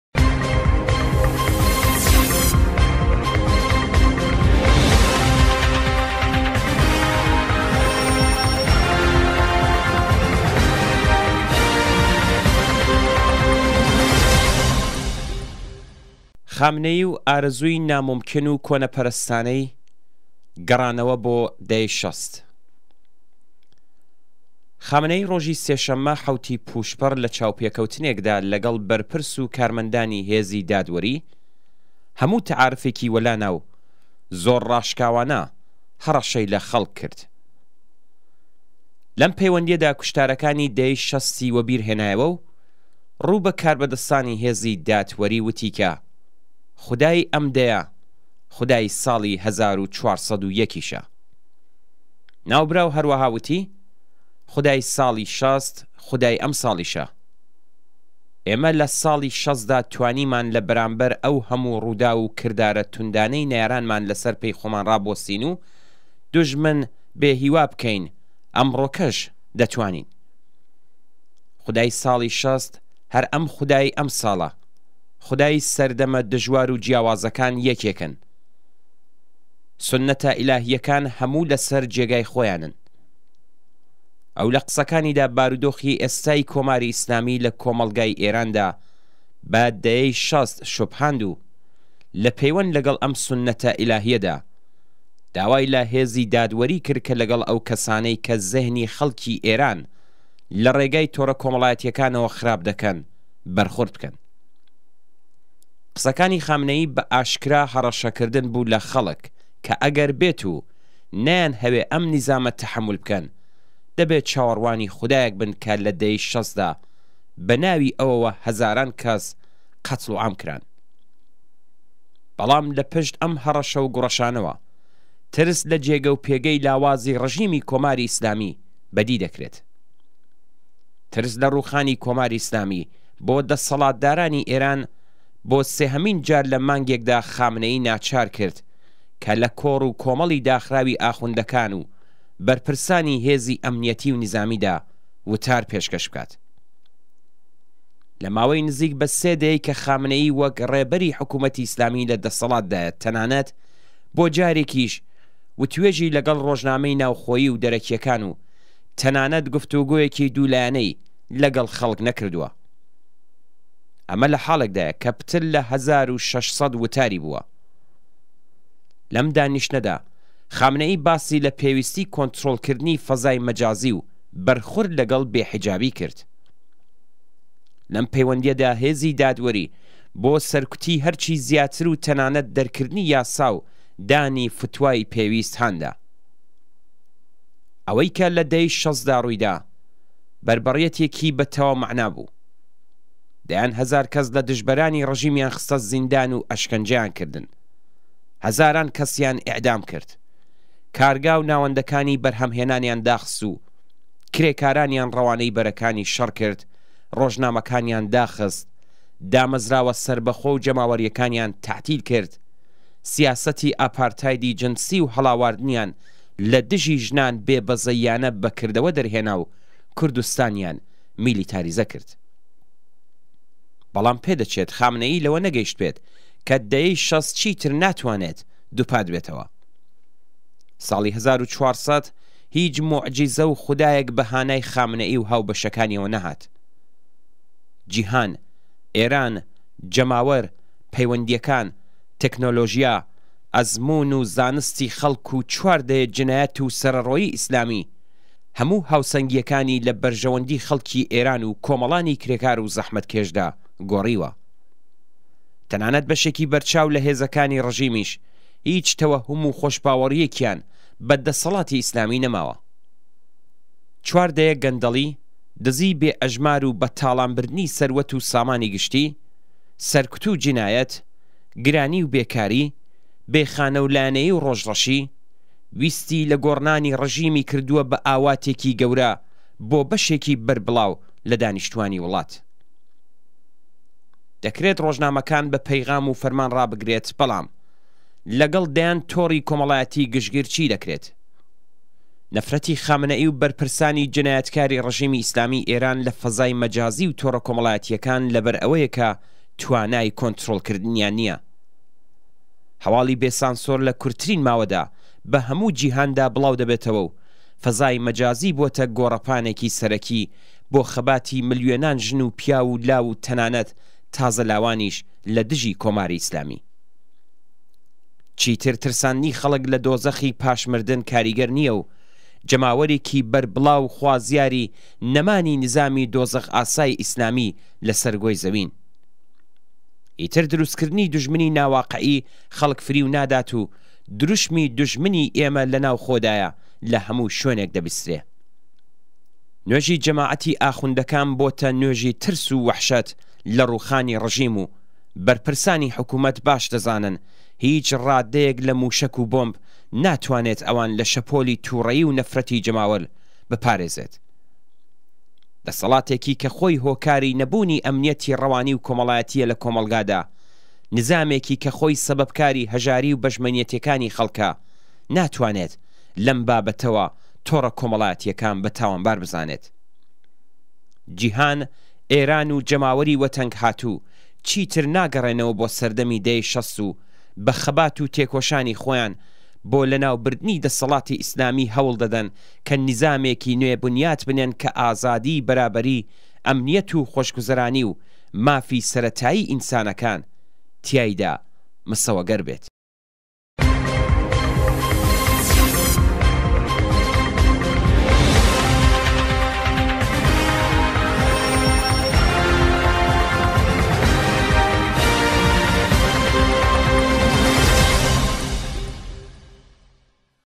وتەی ڕۆژ 12.04.1401
خامنه‌یی و ئاره‌زووی نامومكین و كۆنه‌په‌ره‌ستانه‌ی، گه‌ڕانه‌وه‌ بۆ ده‌یه‌ی شه‌ست